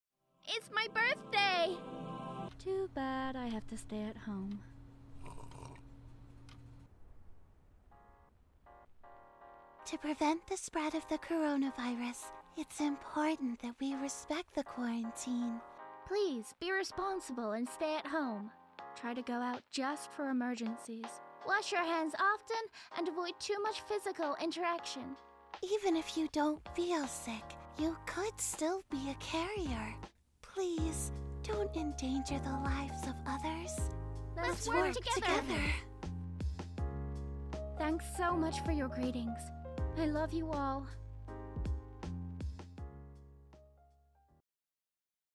Coronavirus Prevention
Happy Birthday Gaghs!
Too bad Coronavirus is affecting everyone, but we can handle it if we are responsible with our actions.
Please follow instructions and stay safe!
Gaghs and Zhima's Voice Actress